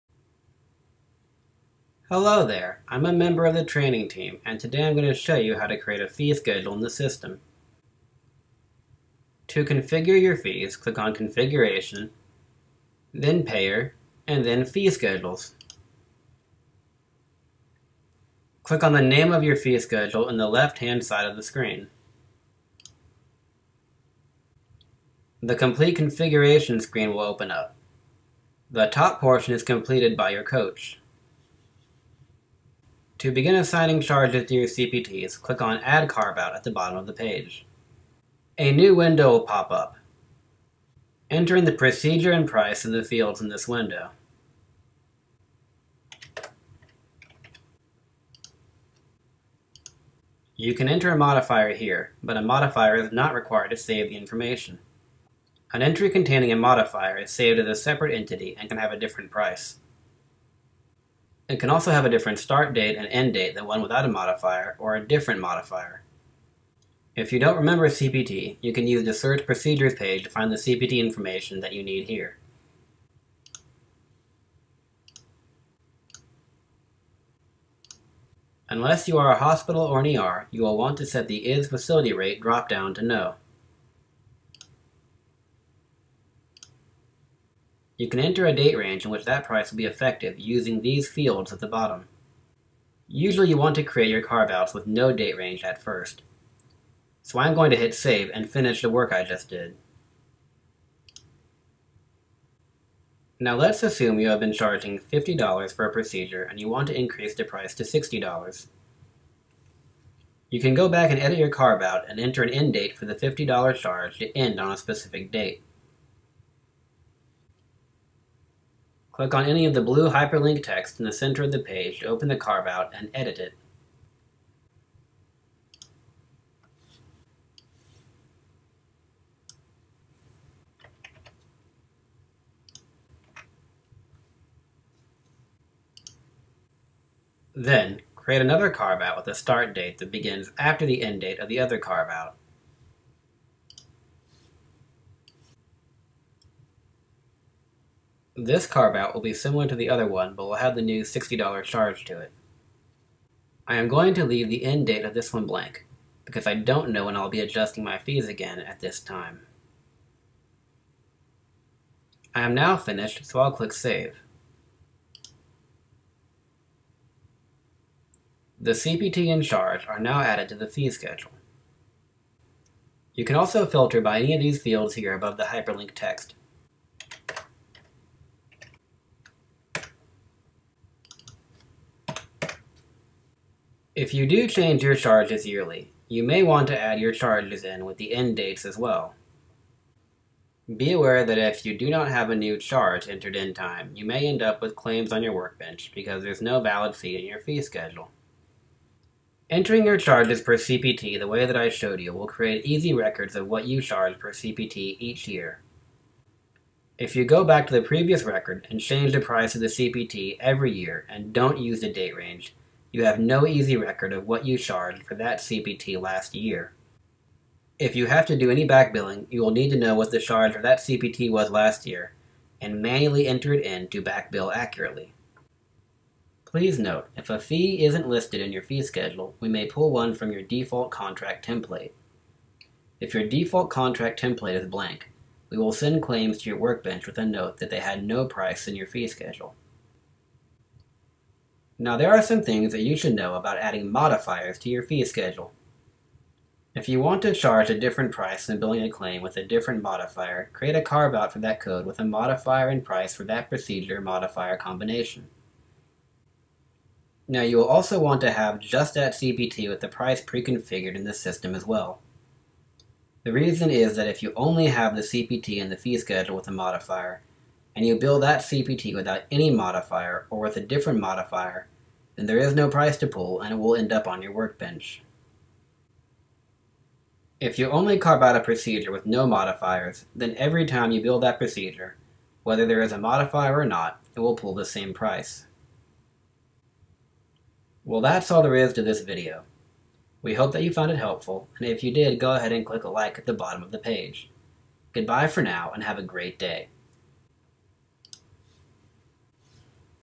Medical Billing and Practice Management Software-Fee Schedule
Vericle products include Practice Management Software, Outsourced Medical Billing, and Practice Owner Coaching services. This video explains how to create a fee schedule in the system. Vericle software is integrated with Genesis Chiropractic Software bestPT and ClinicMind